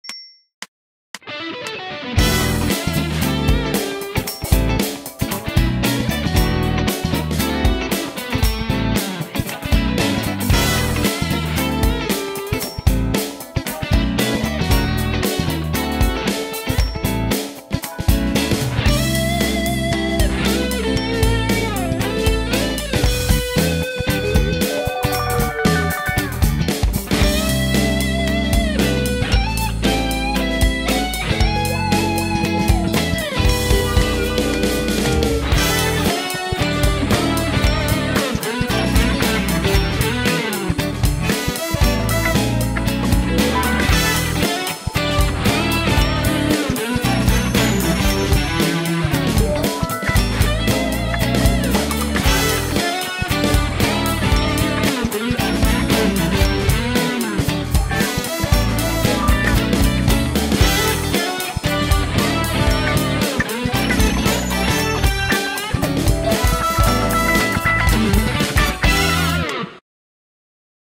Generic Fusion Drum Cover
A short jam over some generic Jazz Fusion from the Roland backing tracks. I didn't want to be that guy who drums with his shirt off, but it was hot as in my practice room!